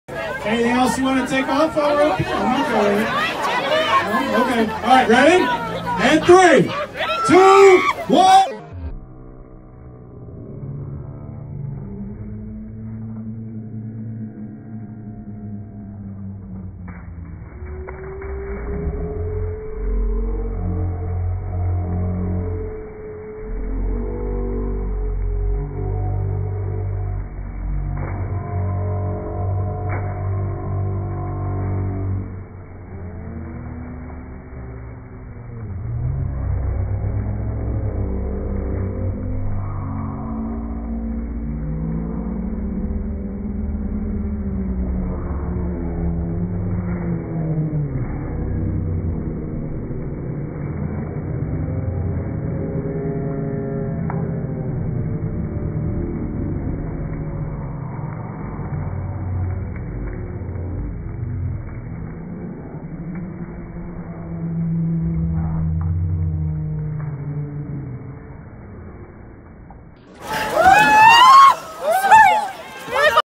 Polar Plunge 2020
The 95 WIIL Rockers take the plunge at the legendary Brat Stop in support of the Special Olympics!